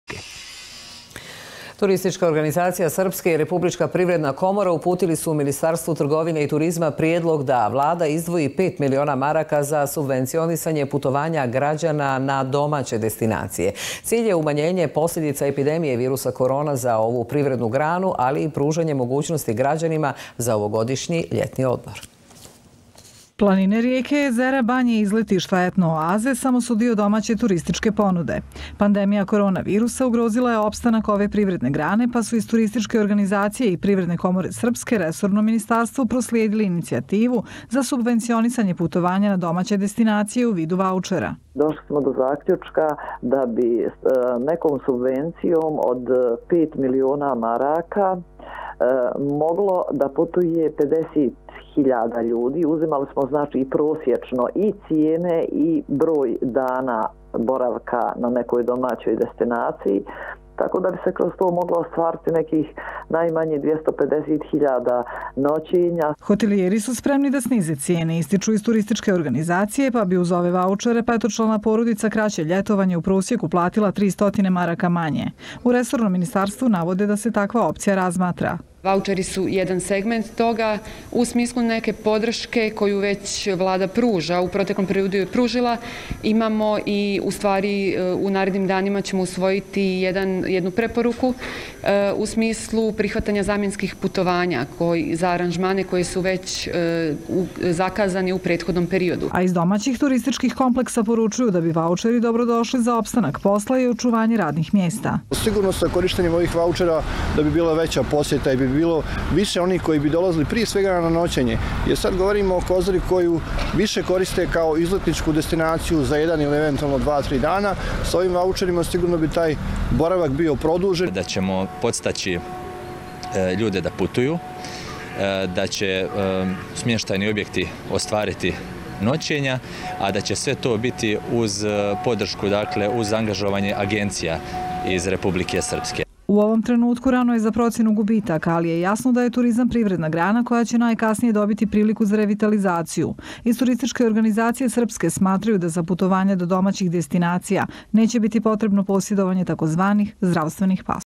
ВАУЧЕРИ КАО МЈЕРА ПОДСТИЦАЈА ОПОРАВКУ ДОМАЋЕГ ТУРИЗМА
Туристичка организација и Првиредна комора Републике Српске упутили су Министарству трговине и туризма РС пријелог за ублажавање негативних посљедица које су услијед пандемије COVID-a задесиле домаћи туризам.
У питању су ваучери којим би се суфинасирало путовање становника Републике Српске на домаће дестинације, а који би представљали мјеру подстицаја у циљу опоравка домаћег туризма.
Опширније у прилогу Дневника 2 РТРС-а: